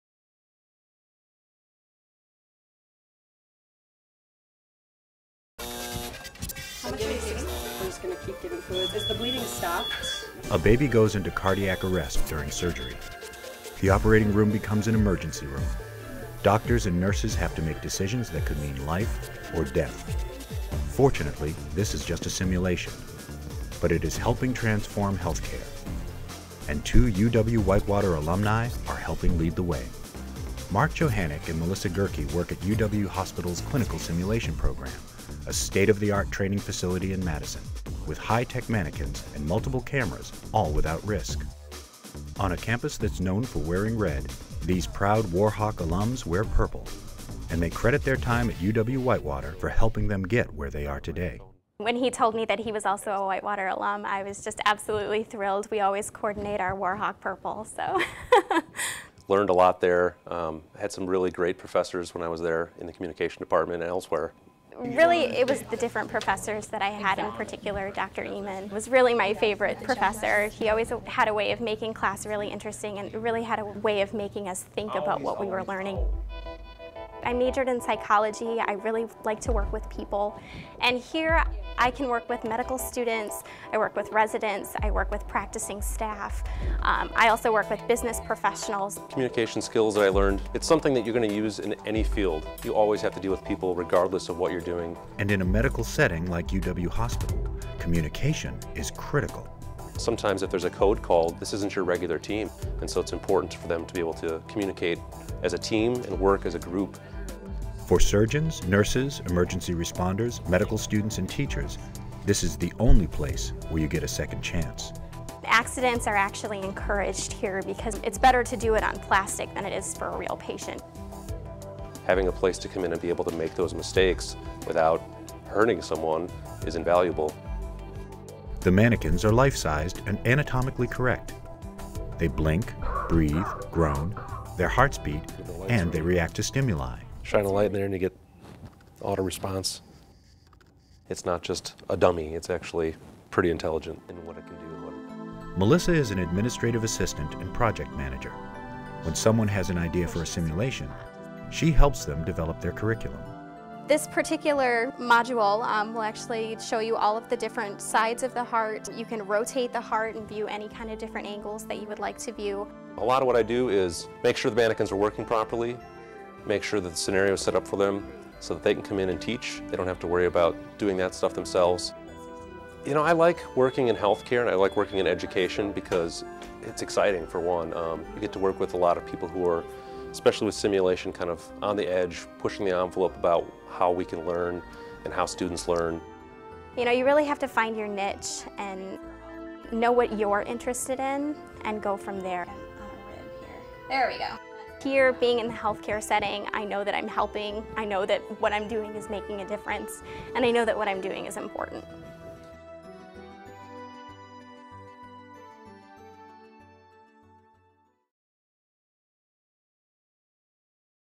Saving Lives Through Simulation
UW-Whitewater alumni Mark Johanneck and Melissa Gerke work at UW Health Clinical Simulation Program in Madison, WI. They credit their UW-Whitewater education with helping them get where they are today.